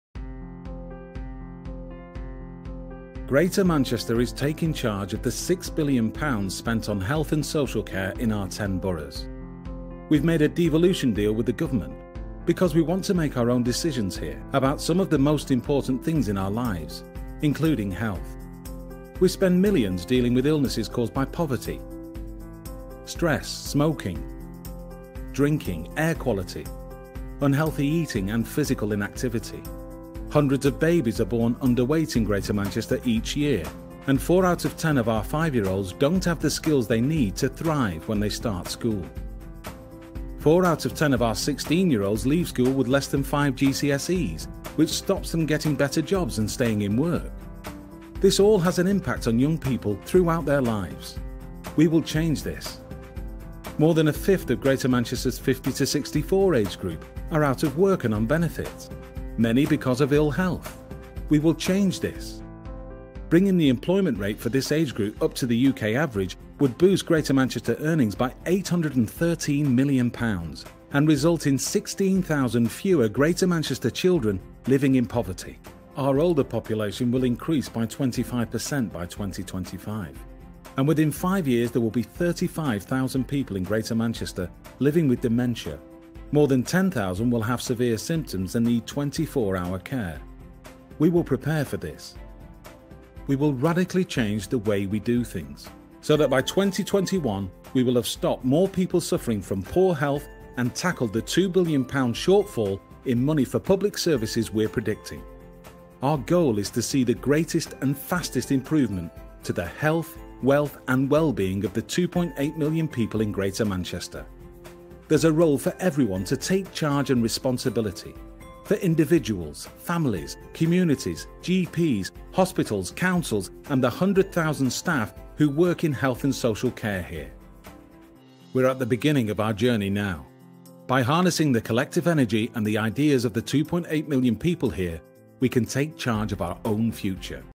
Greater Manchester Health and Social Care Devolution
We have produced a short, two minute animated film to provide a  summary our five-year vision for Greater Manchester. Using four colourful characters to represent the GM community life stages (baby, teenager, working man and elderly grandmother), it has been produced with or without subtitles